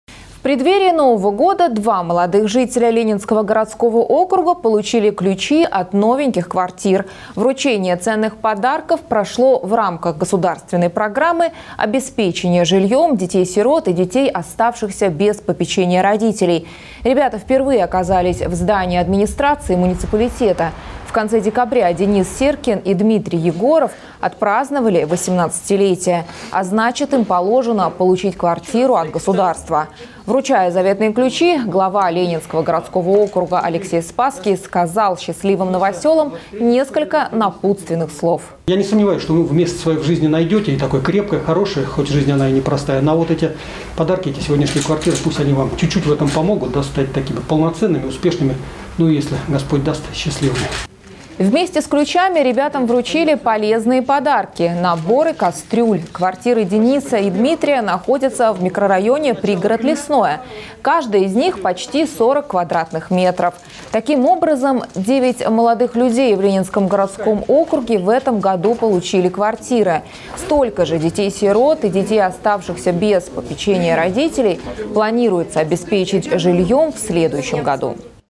«Хороший старт» - вручение ключей двум юным жителям Ленинского городского округа
В преддверии Нового года два молодых жителя Ленинского городского округа получили ключи от новеньких квартир. Вручение ценных подарков прошло в рамках государственной программы обеспечения жильем детей-сирот и детей, оставшихся без попечения родителей.